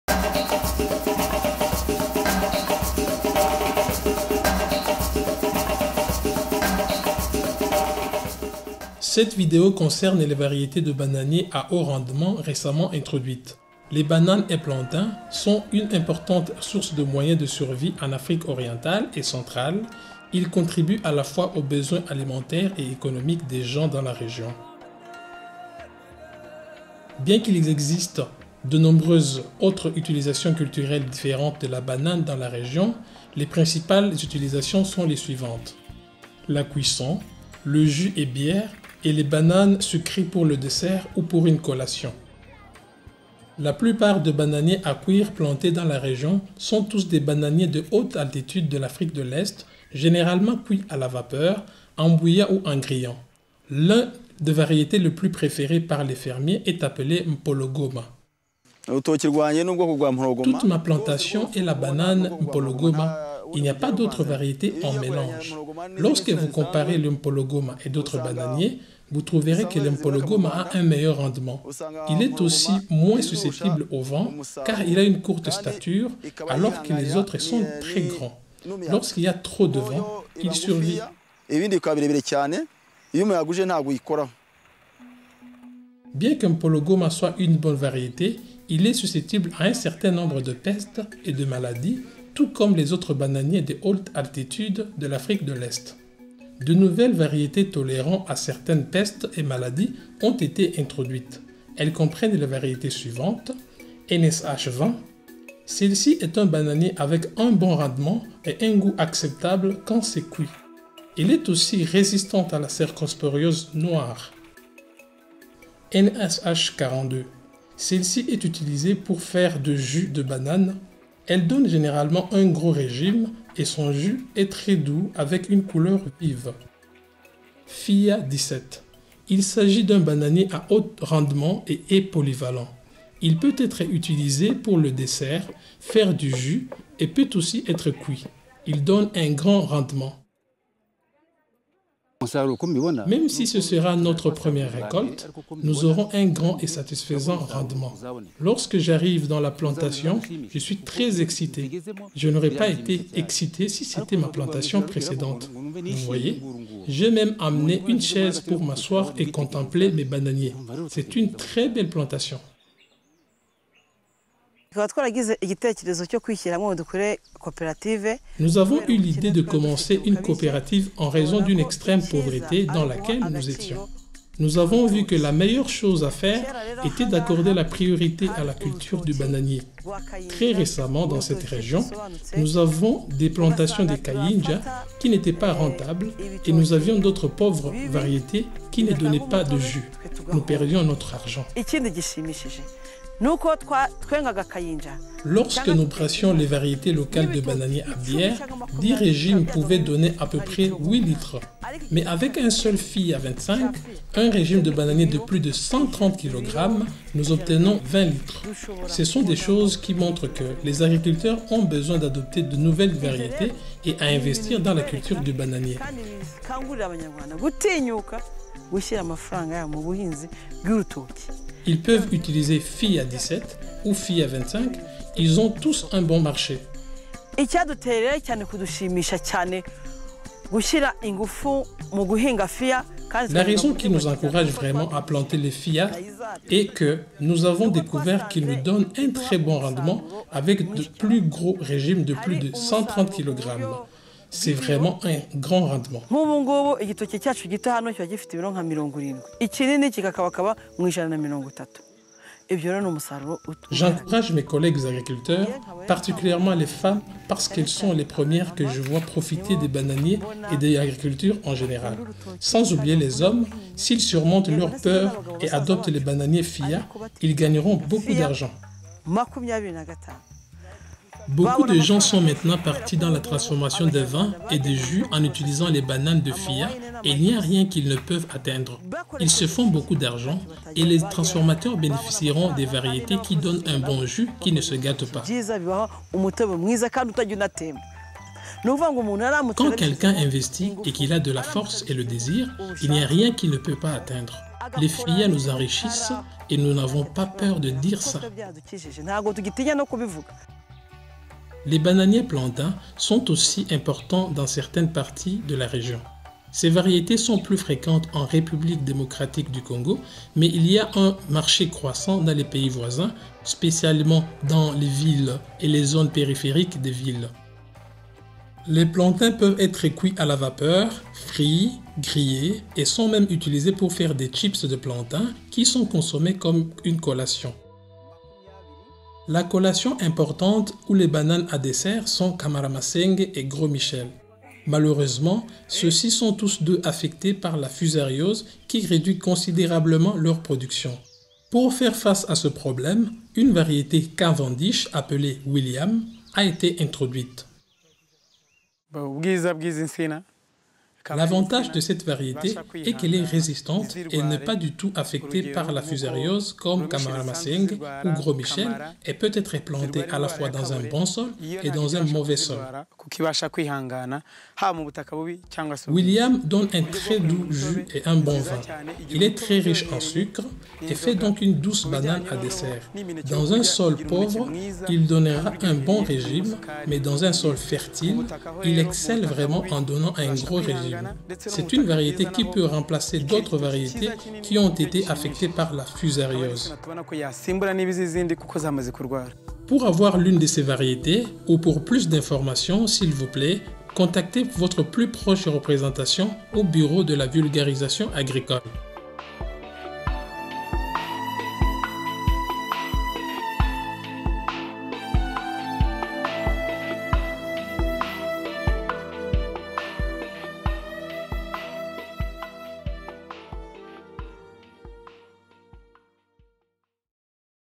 video training for Africa . varietés de bananes | FR
Présentation de variétés de bananes à haut rendement.

Cette vidéo fait partie de la série 'Video Training for Africa' réalisée pour la KULeuven et Cialca, un consortium qui veut améliorer les conditions des agriculteurs en Afrique centrale.

Een film van | Un film de | A film by

Sophie Spillemaeckers
Ludovic Schweitzer